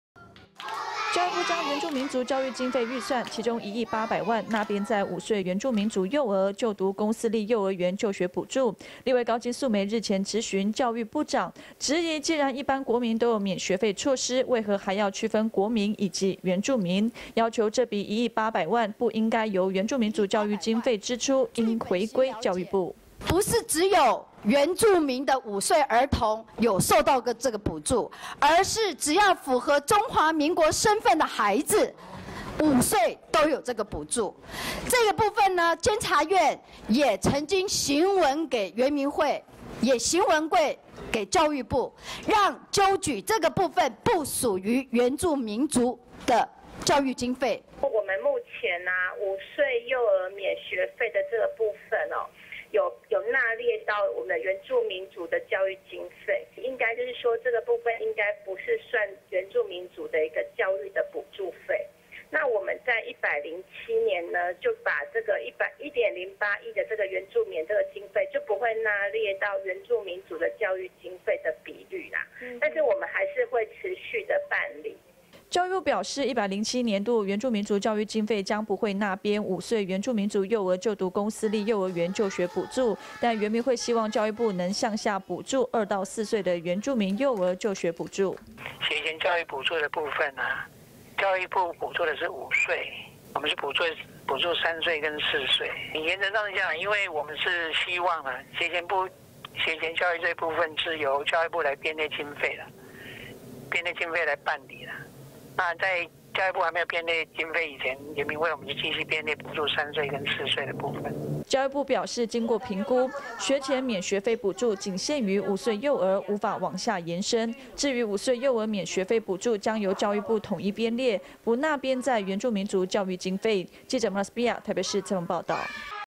原住民5歲幼兒免學費 經費編列回歸教部 2017-05-08 TITV 原視新聞
教育部將原住民族教育經費預算，其中1億800萬納編在5歲原住民族幼兒，就讀公私立幼兒園就學補助，立委高金素梅日前質詢教育部長，質疑既然一般國民都有免學費措施，為何還要區分國民及原住民?要求這筆1億800萬不應該由原住民族教育經費支出，應回歸教育部。

教育部表示，107年度原住民族教育經費，將不會納編5歲原住民族幼兒就讀公私立幼兒園就學補助。但原民會希望教育部，能向下補助2到4歲的原住民幼兒就學補助。

教育部表示經過評估，學前免學費補助僅限於5歲幼兒，無法往下延伸，至於5歲幼兒免學費補助將由教育部統一編列，不納編在原住民族教育經費。